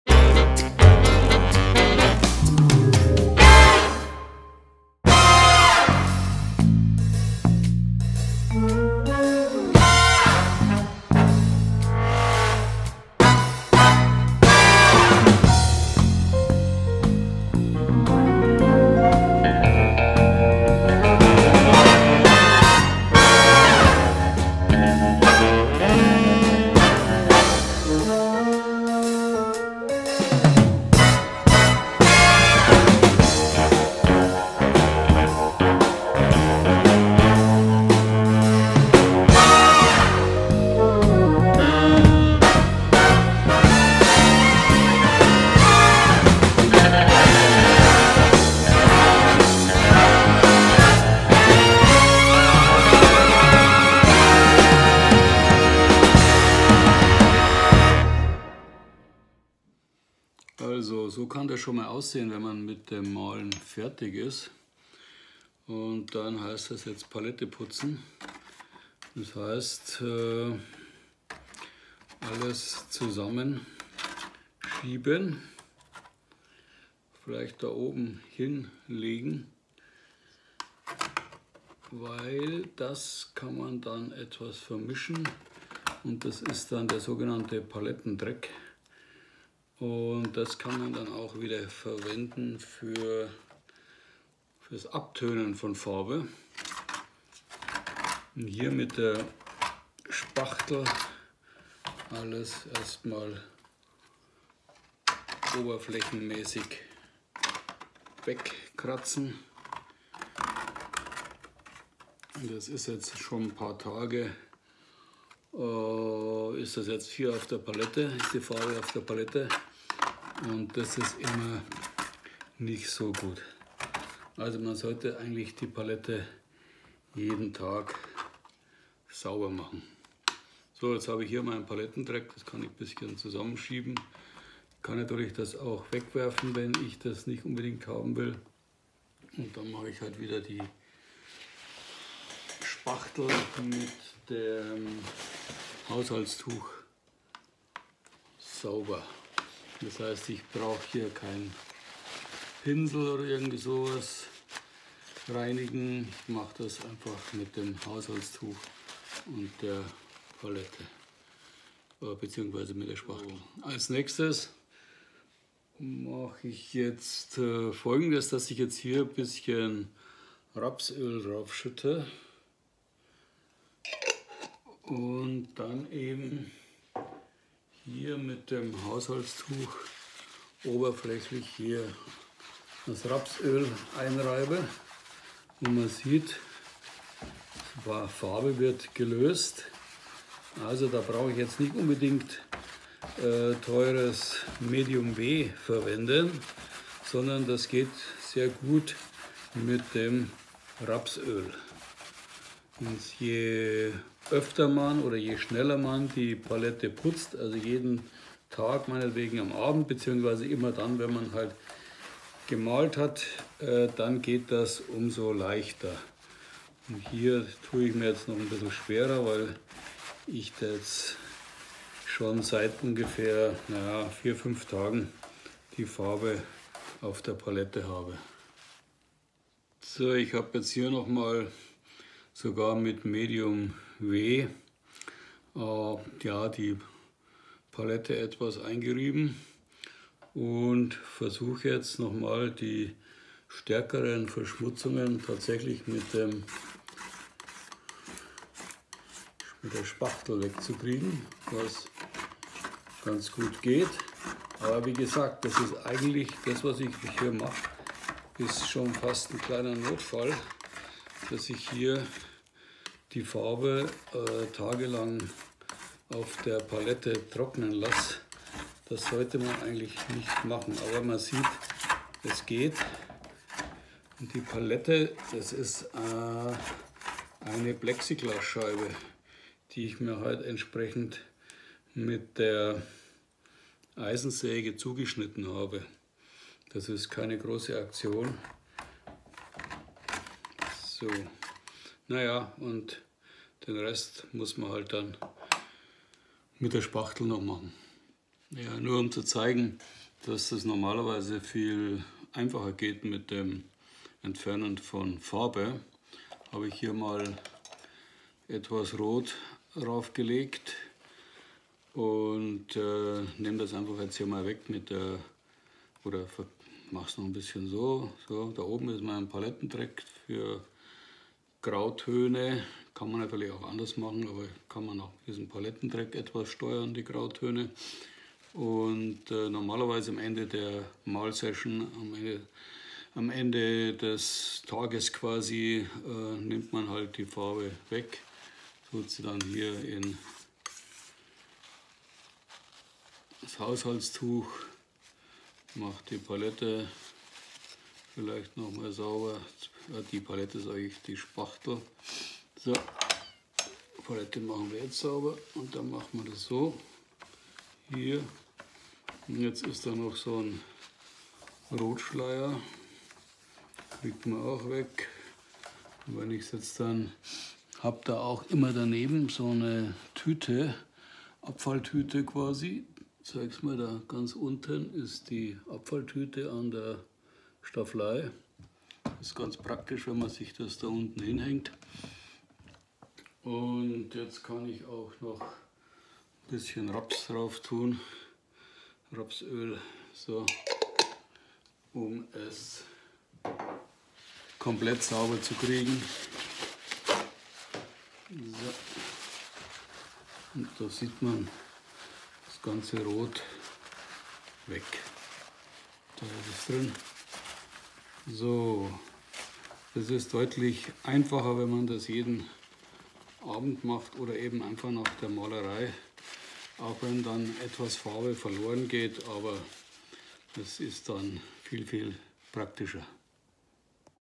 Mit Ölfarben malen, leicht gemacht – Palette putzen
Er wird gezeigt, wie man OHNE Verwendung von Lösungsmitteln, sei es Terpentin, Shellsol etc. die Palette putzen kann. Entweder verwendet man Medium W oder Rapsöl aus dem Supermarkt oder Tante-Emma-Laden.